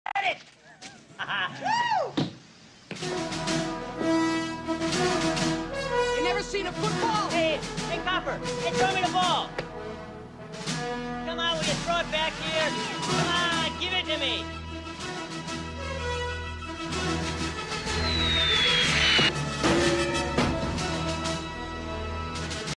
Bubba Smith Rememberance Video
Just a short video to the late Bubba Smith using various videos floating around youtube..